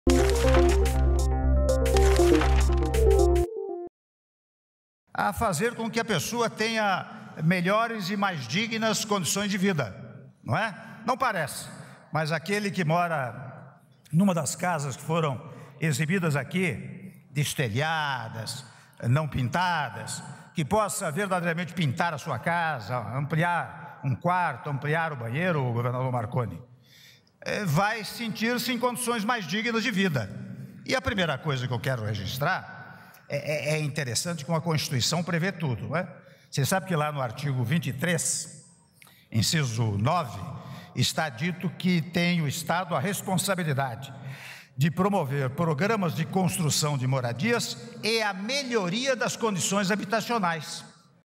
Cartão Reforma irá melhorar condições de vida dos beneficiários, diz presidente Temer
EM DESTAQUE - 09.11.16: Nesta quarta-feira (9), o governo federal lançou o Cartão Reforma, que libera até R$ 5 mil em crédito para famílias reformarem moradias. Durante cerimônia, presidente Michel Temer disse que fará com que beneficiários tenham condições mais dignas de vida.